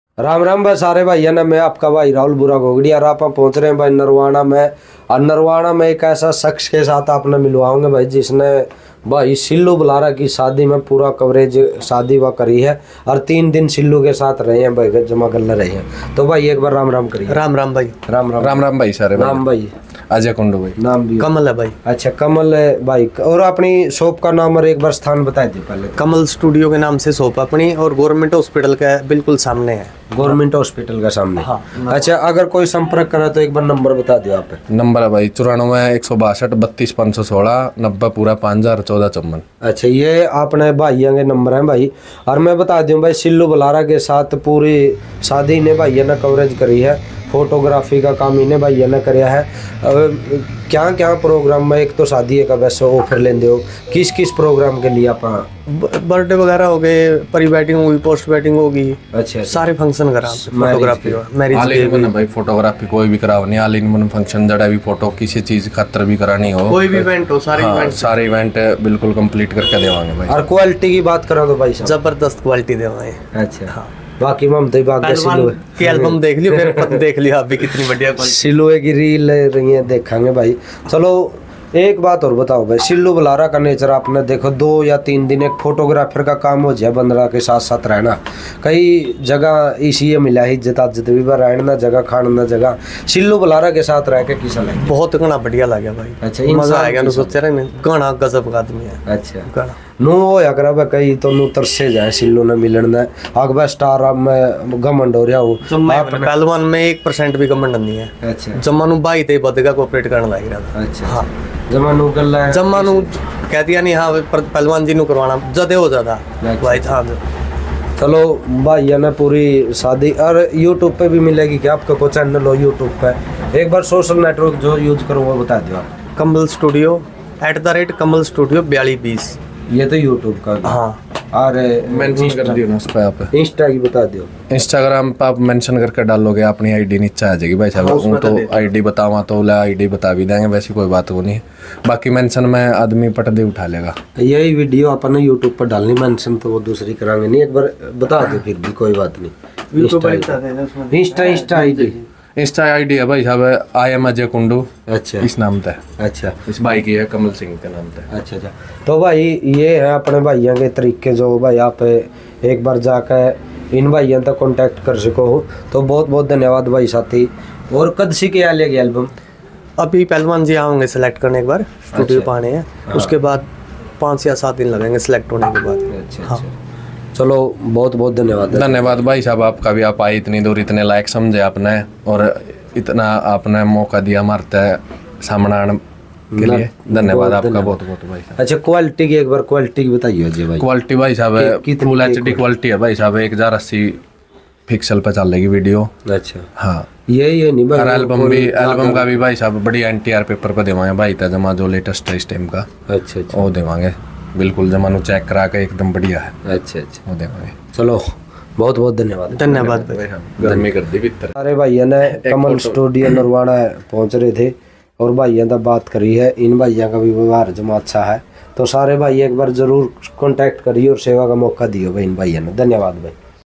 शीलू बल्हारा की शादी कवरेज करने वाले भाईयो ने शीलू का व्यावहार कैसा बताया आप भी जरूर सुनो क्या कहा
शीलू बल्हारा की शादी SHILU WITH MUSKAN वर माला की रस्म देखो शीलू को ऊपर उठा लिया
शीलू बल्हारा का जन्म हुआ था जिस मे वो घर देखो शीलू की दादी ने क्या कहा